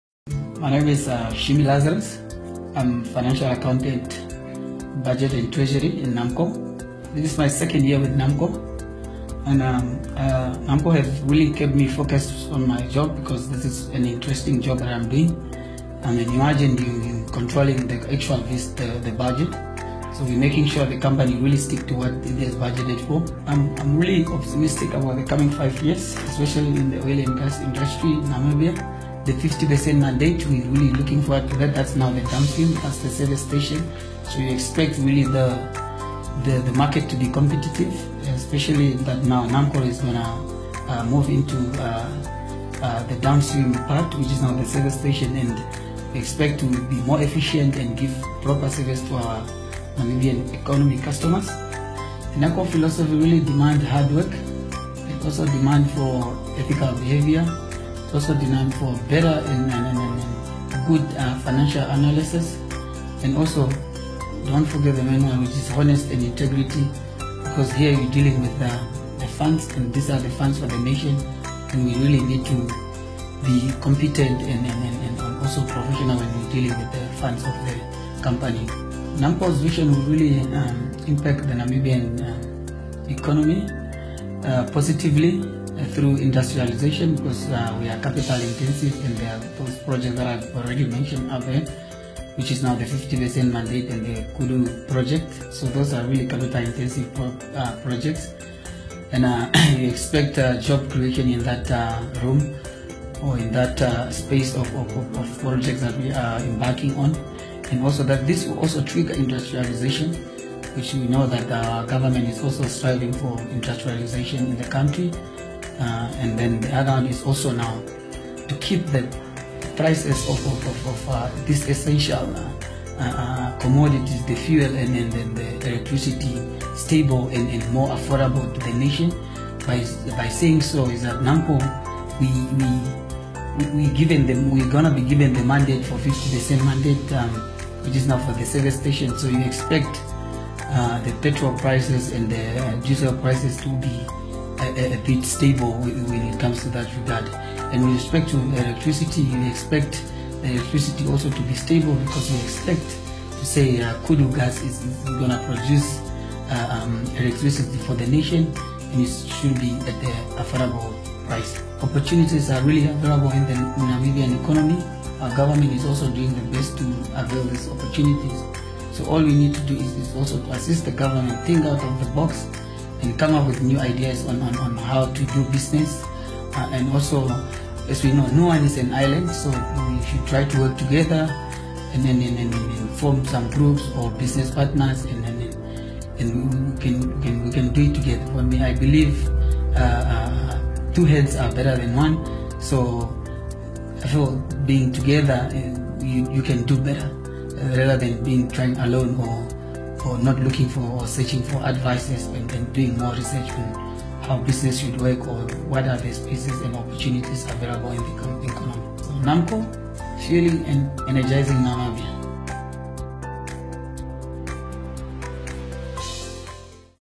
Lazarus Shiimi aka Gazza energising Namibia
Lazarus Shiimi is the financial budgeter and accountant at Namcor Namibia and is making majour impactbin terms of using the parastate's finances sparingly and efficiently.